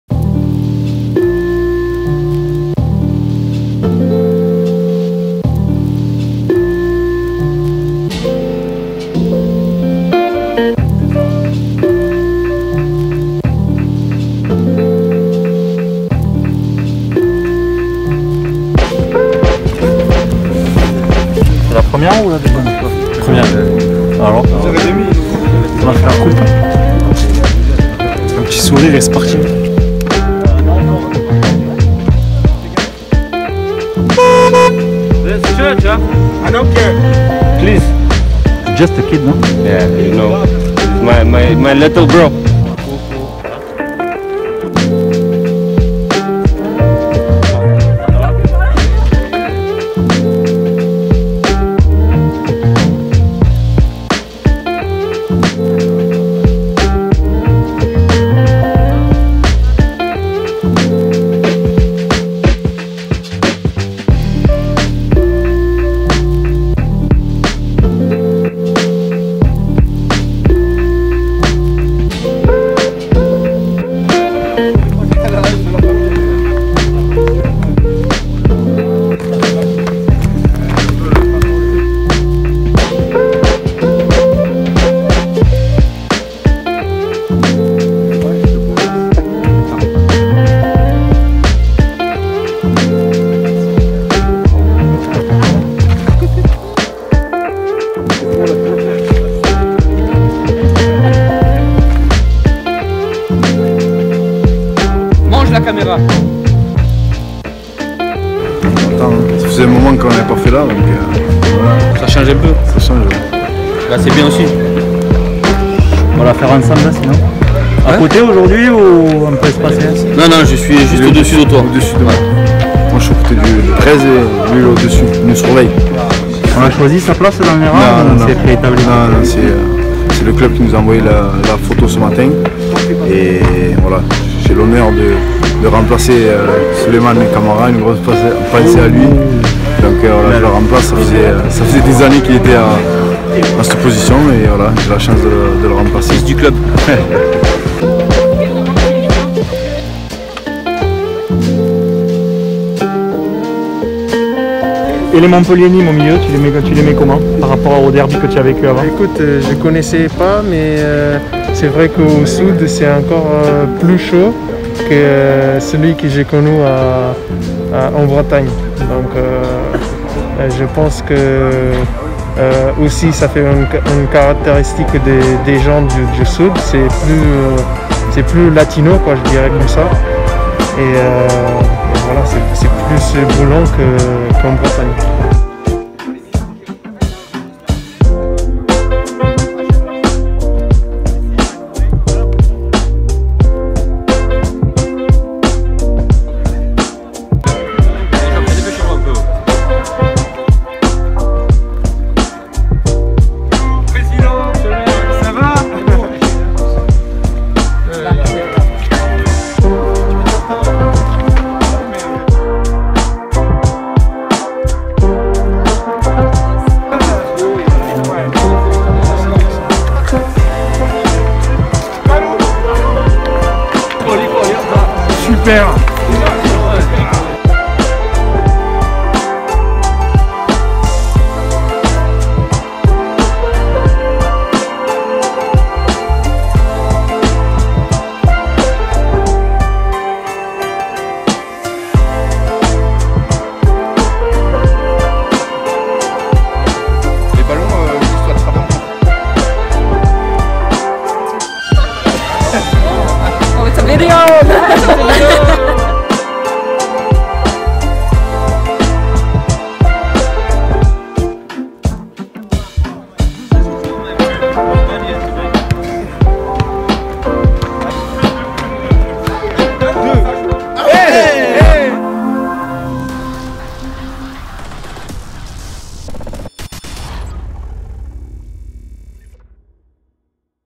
La photo officielle saison 2020/21 !
Photo officielle des pros et de la D1F du MHSC tirée ce mardi en fin de matinée au parc de Grammont.